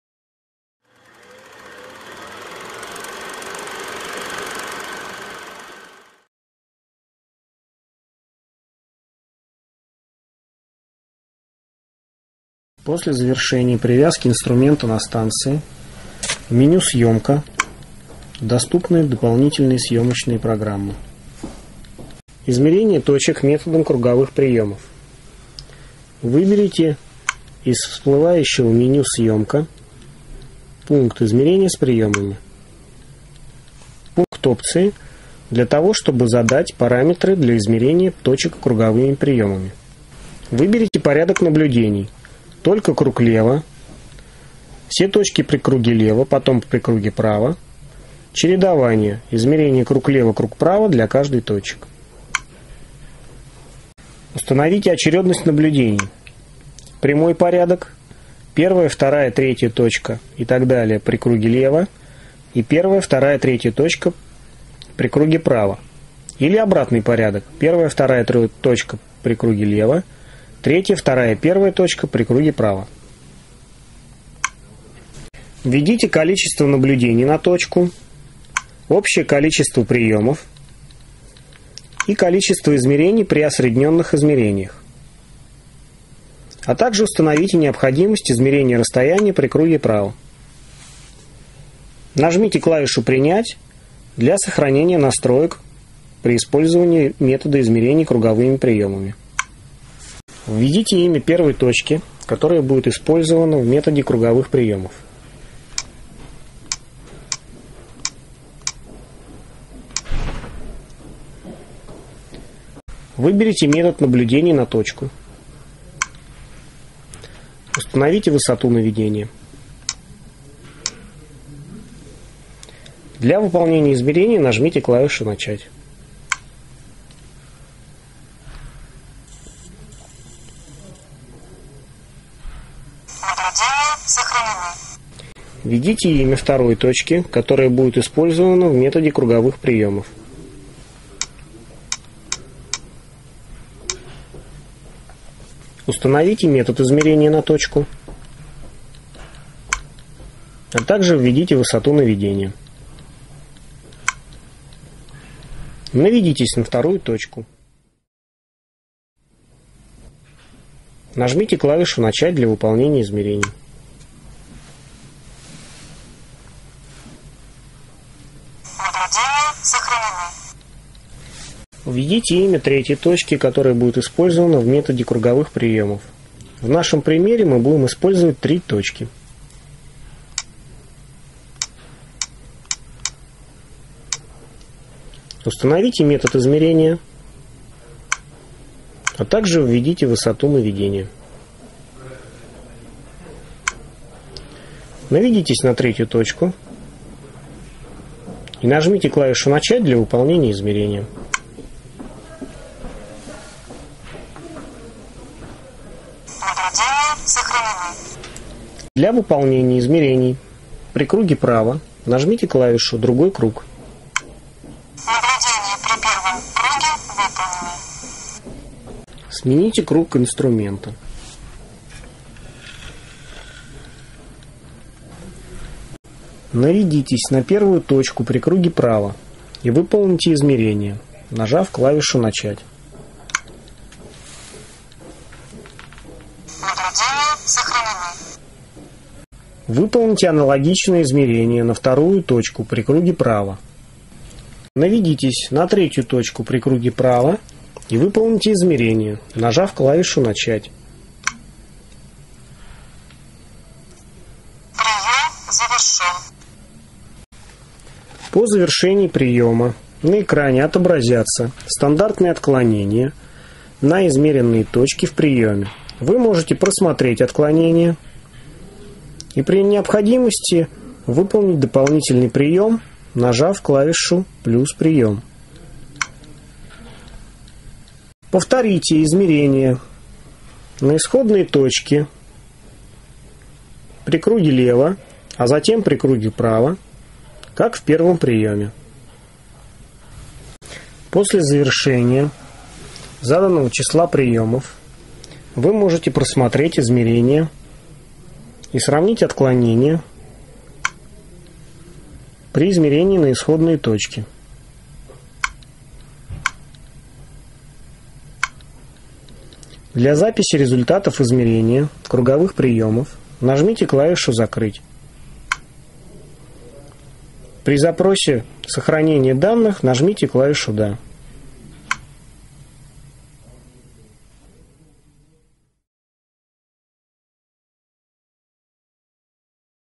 Измерение точек методом круговых приемов Trimble M3
Trimble M3